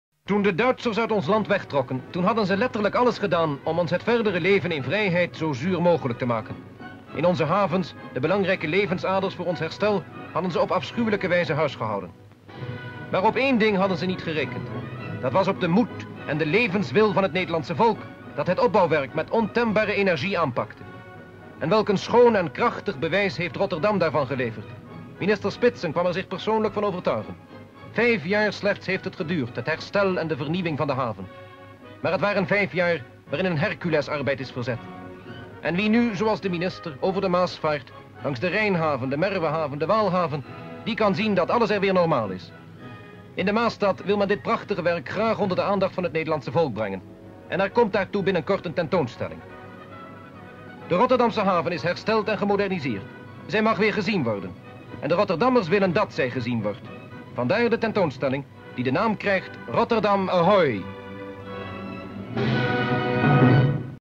The devastated Rotterdam harbour after the Second World War
The devastated Rotterdam harbour after the Second World War, and the port totally reconstructed through much hard work, minister Spitzen takes a look.